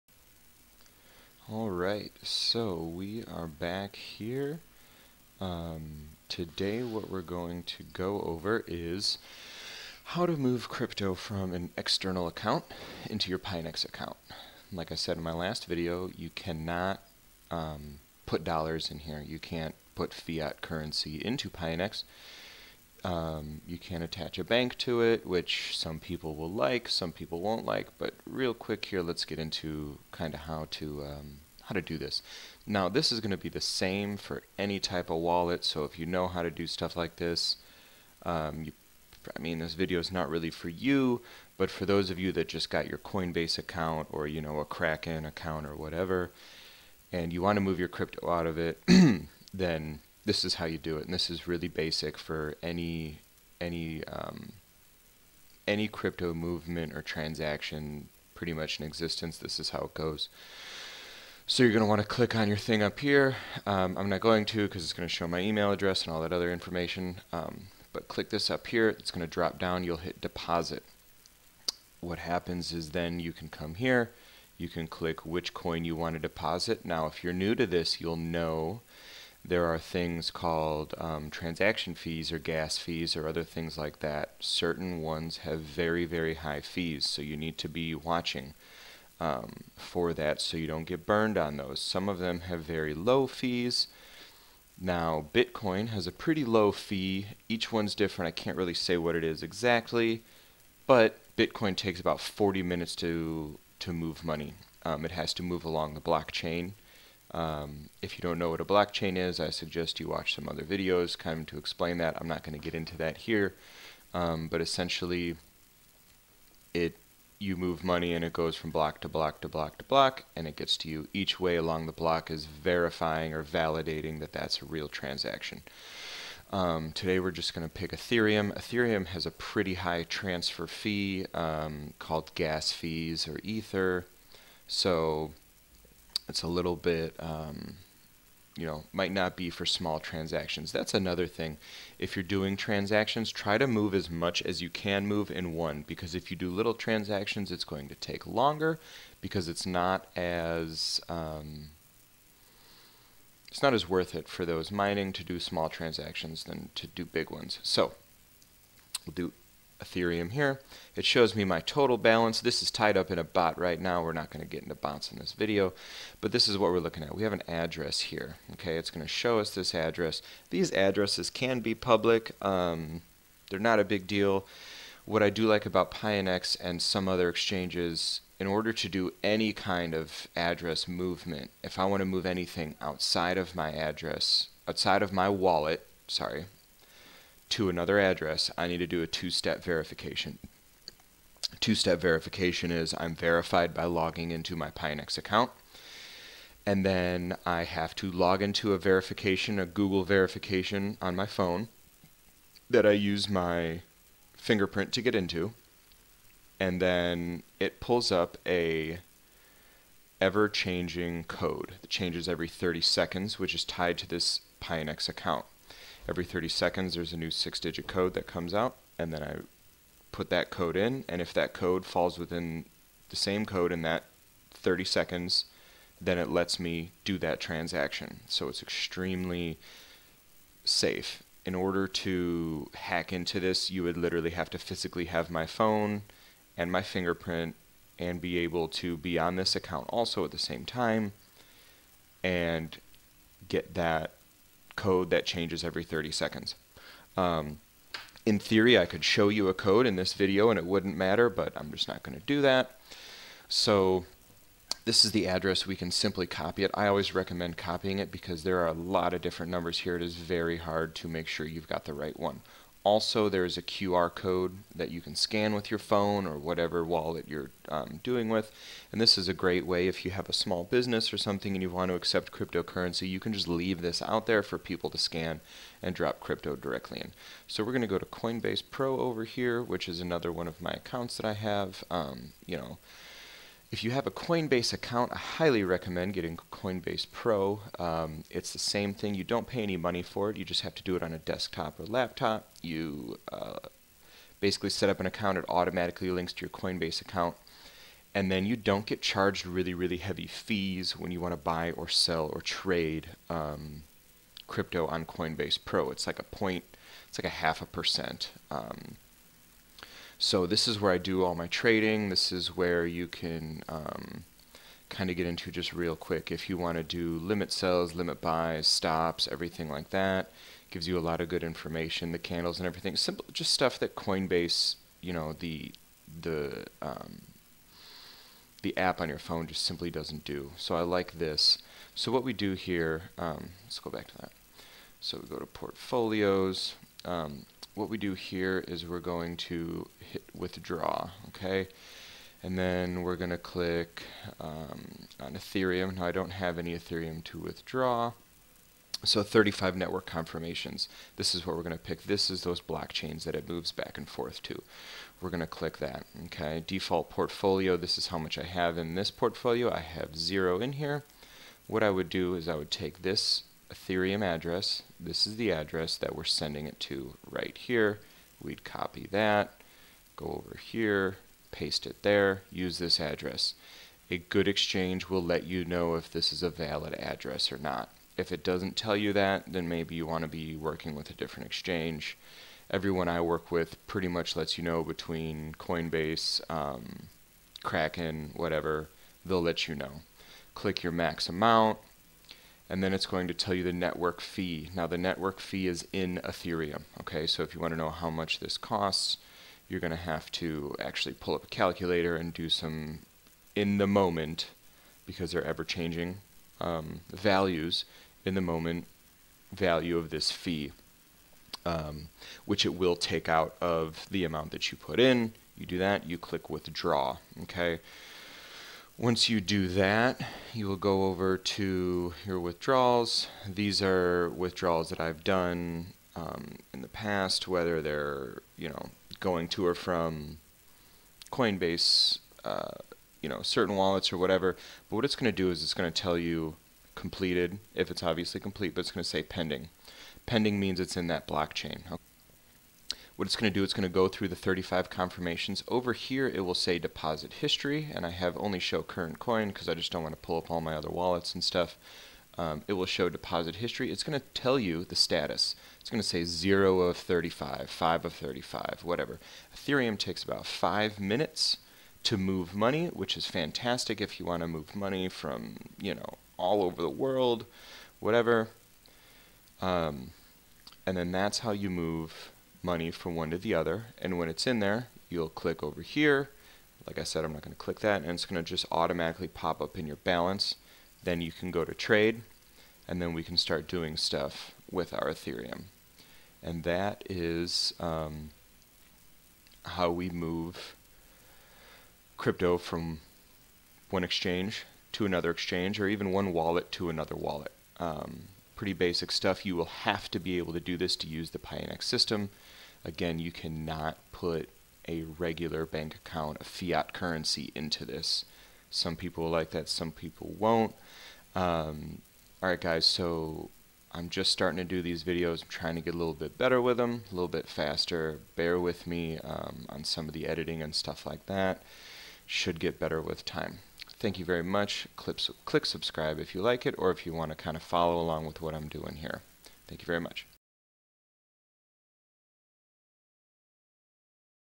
How to move crypto from wallet to wallet using Coinbase Pro and Pionex
This video goes over some of the basic concepts needed when dealing with crypto and how to move your coins from one wallet to another or in this case exchange wallet to another. Still have the filmora watermark as of now.